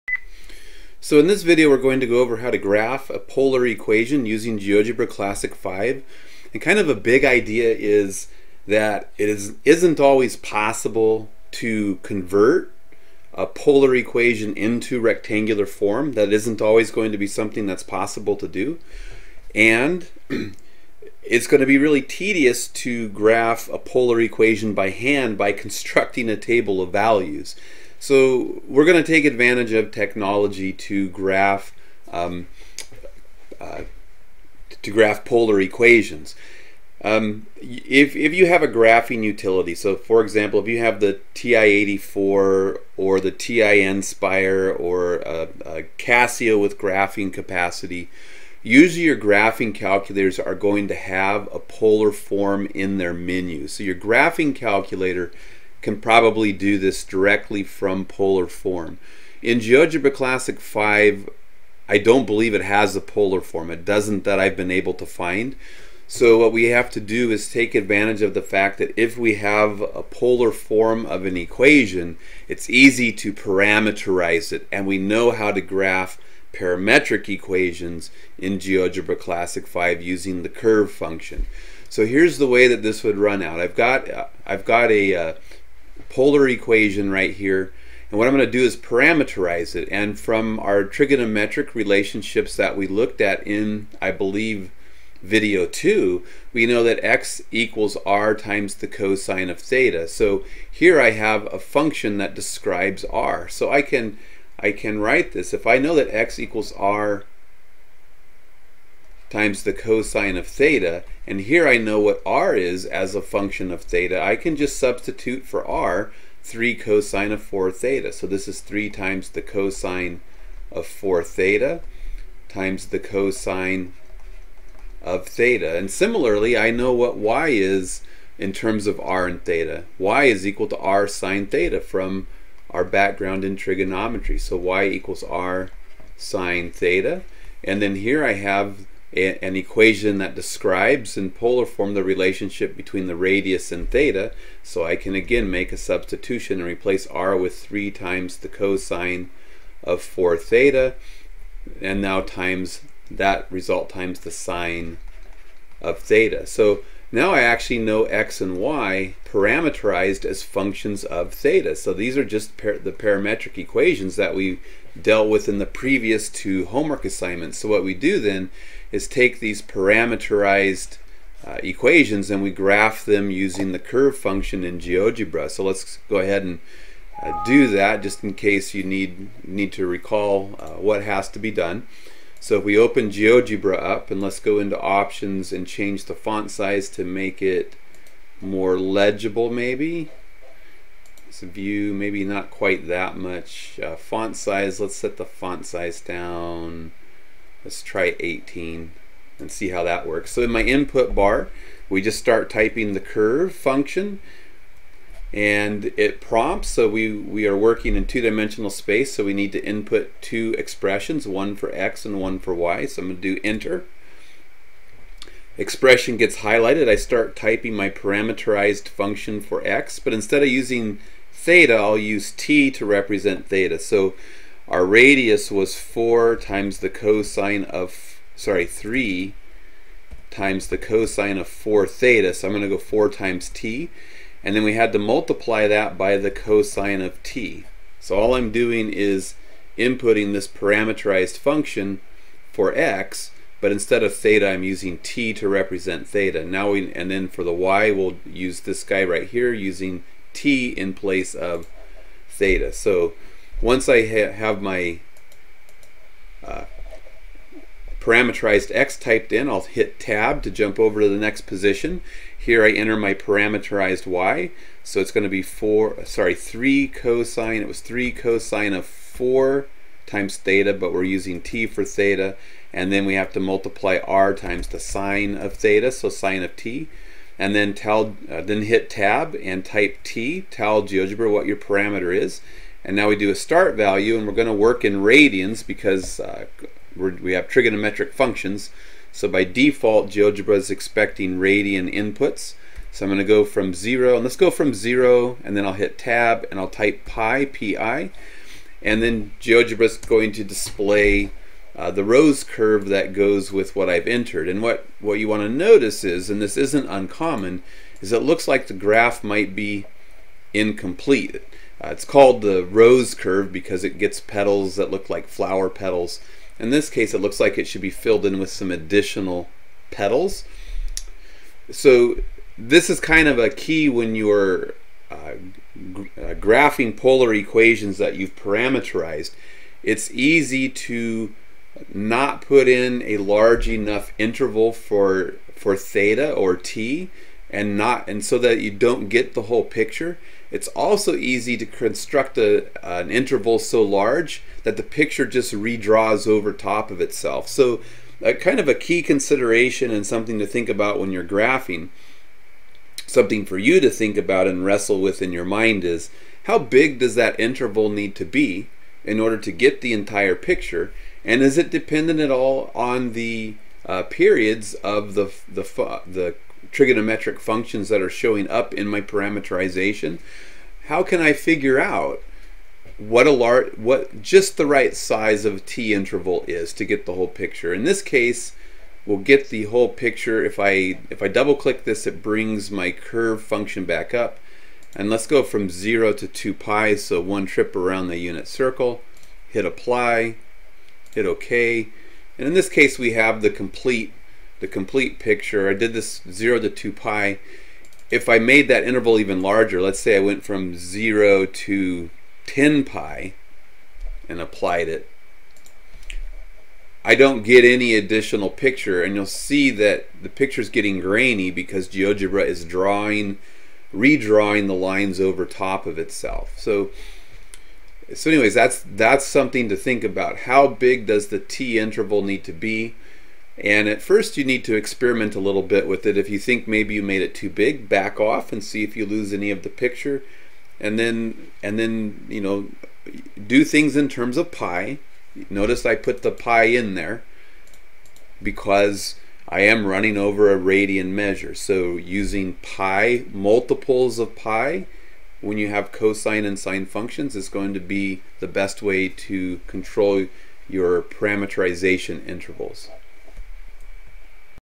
Graphing a Polar Equation Using GeoGebra Classic 5
In this video, we parameterize a polar equation so that we can graph it in GeoGebra Classic 5.